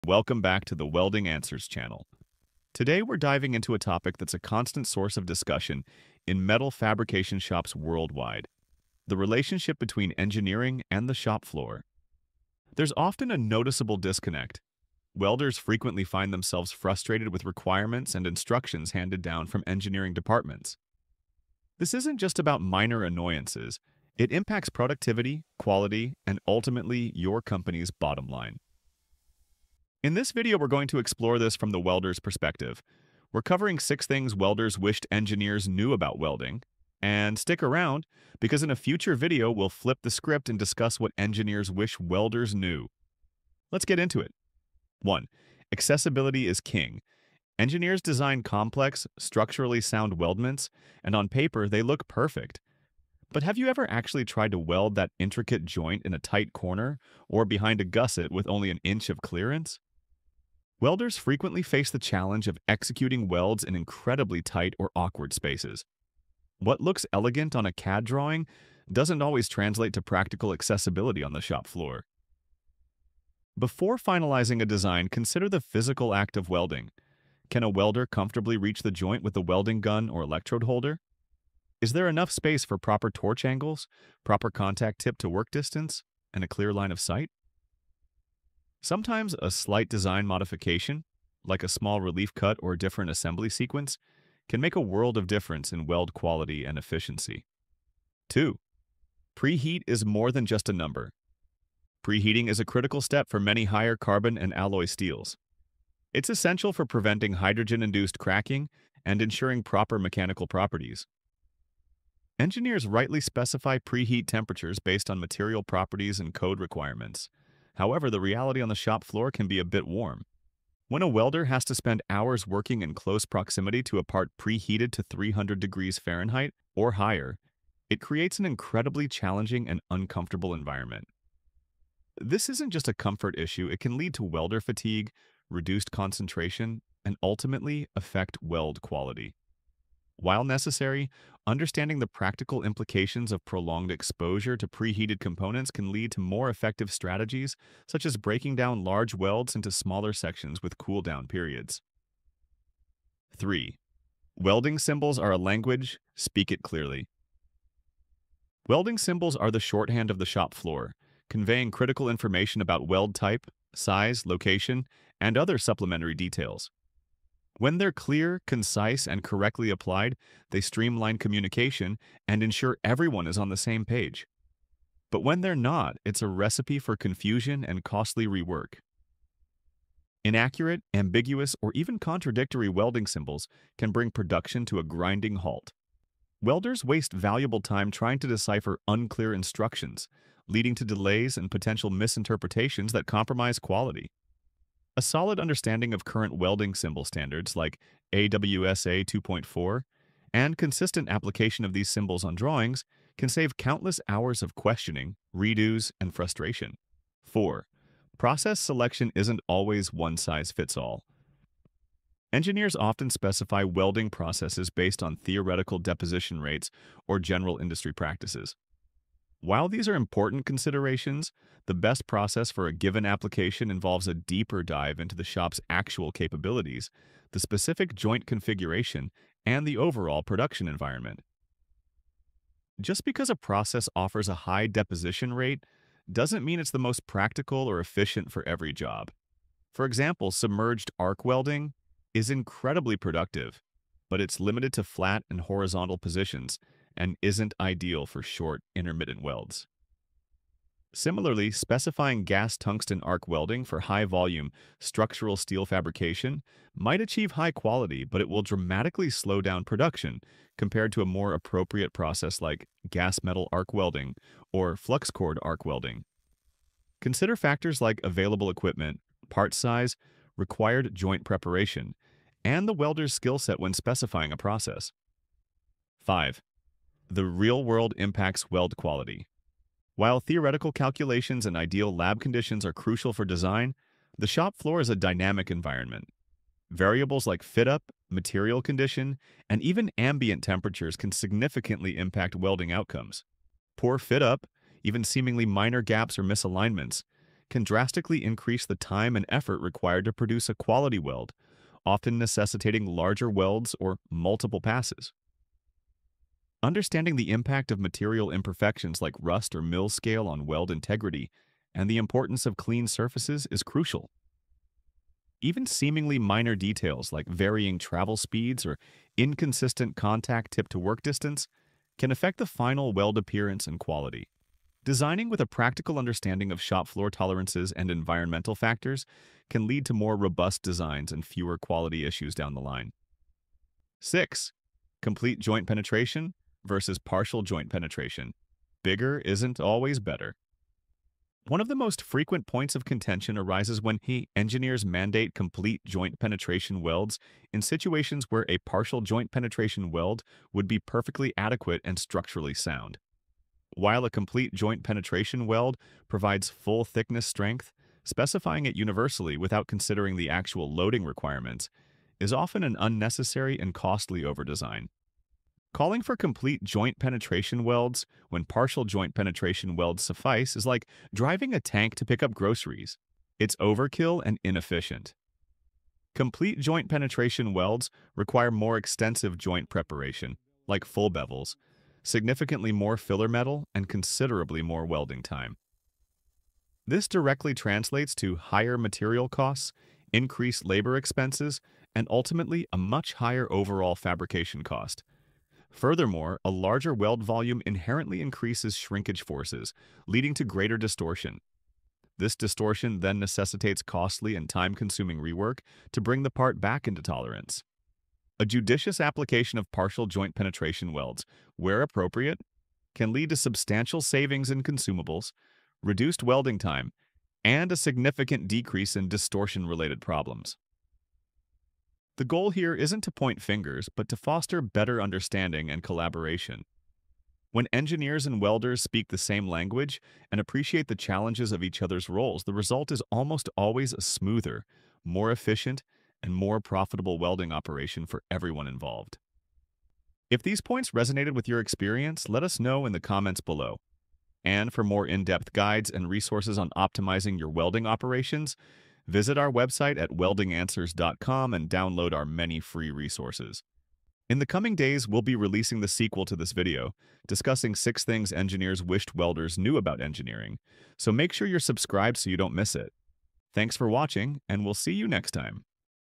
6 Things Welders Wish Engineers Knew About Welding
👉 Free Technical Guides and Other Resources

📌 Video Summary:
There's an age-old disconnect between the engineering department and the shop floor, especially in companies where welding is a core process. Welders constantly face challenges and frustrations stemming from design requirements that seem out of touch with practical realities. From impossible-to-reach weld locations to specifying costly, unnecessary procedures, these issues directly impact productivity, quality, and your bottom line.